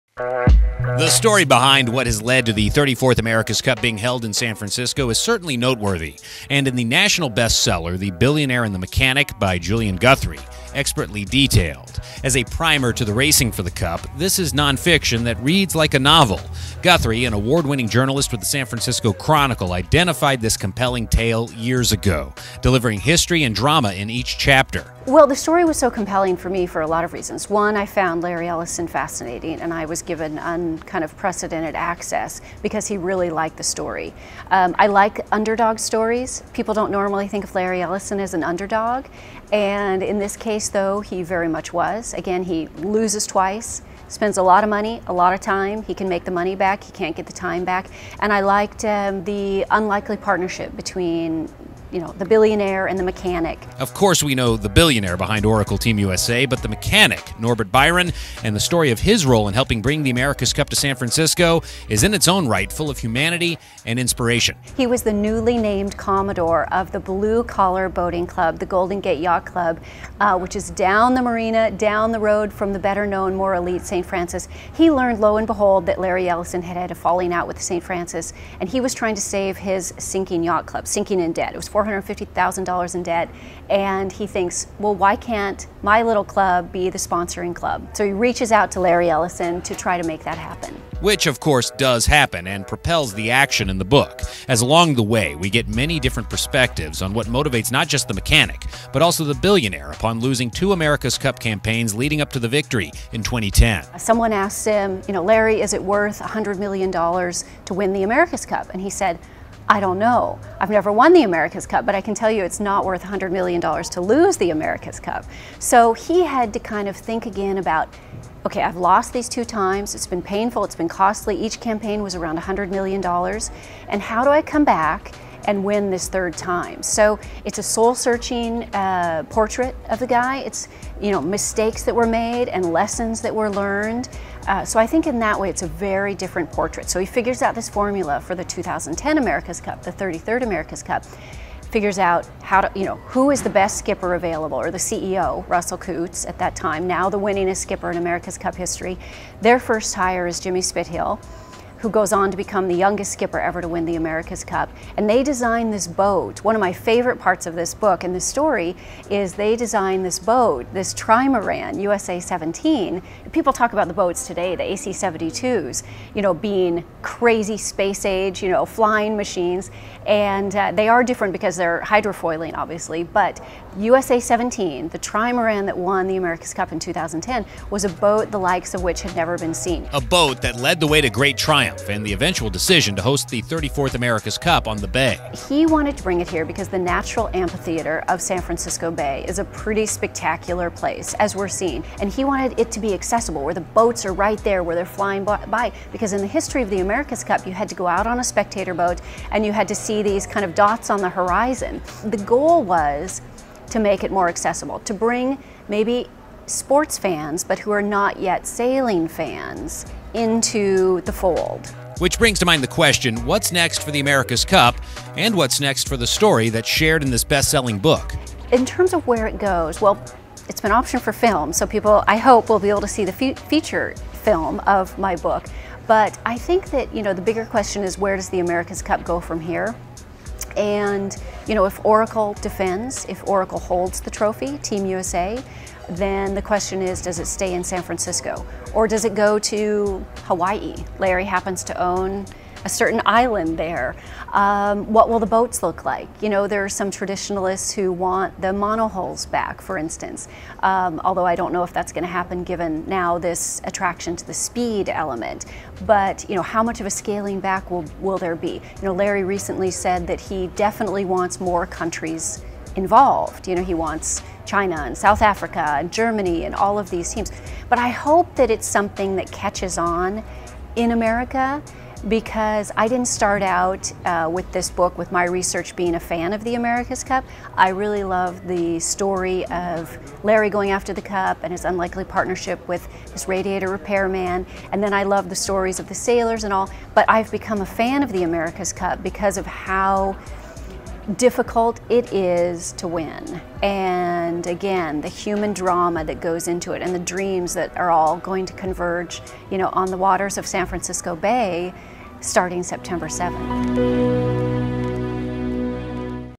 The Billionaire and The Mechanic
Julian Guthrie's best-selling book 'The Billionaire and The Mechanic' details the history of how the America's Cup made its way to San Francisco.